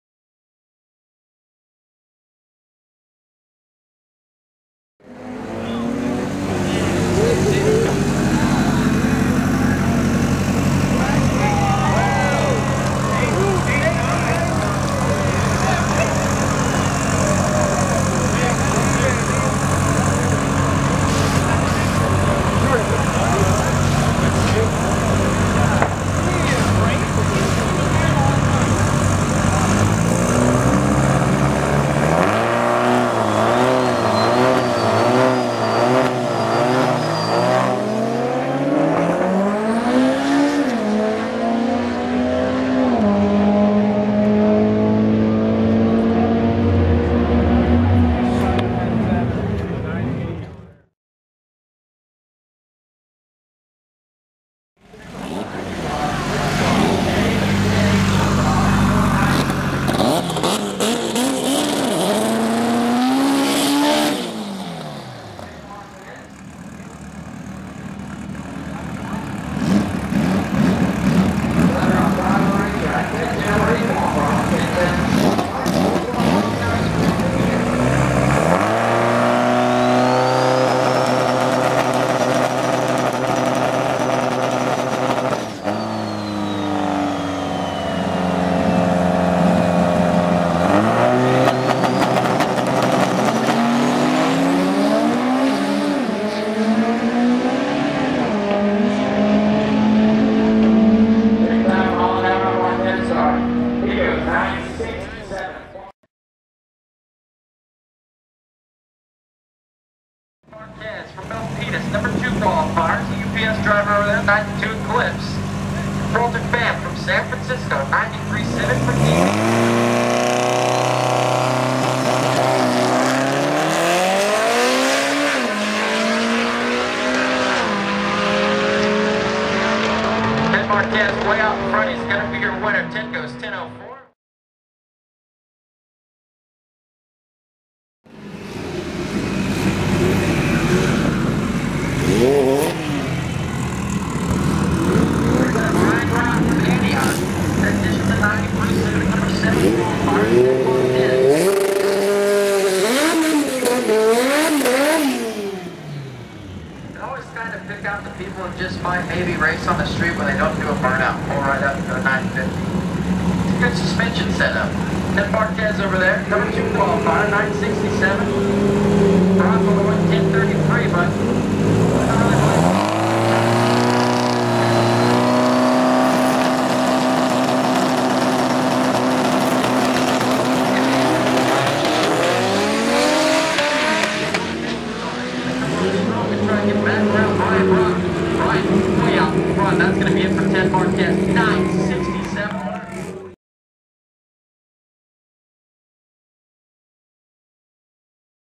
CMI Spring Jam 4/7/2013 Sacramento Raceway 1080p HD
Finally got things settled and dialed in. 4 solid passes figuring out the right combination of launch settings with n20 and the 2step. Slow 60 foots because well. I'm tired of breaking shit.